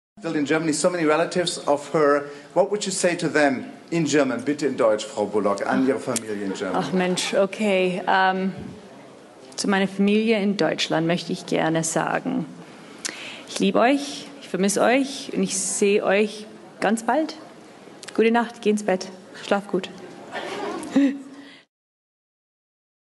Sandra Bullock speaking German at the Academy Awards 2010
ach, Mensch XD

translation:

"Oh boy, okay. To my family in Germany I would like to say: I love you, I miss you. And I'll see you soon. Good night. Go to bed. Sleep well."

video courtesy of wireimage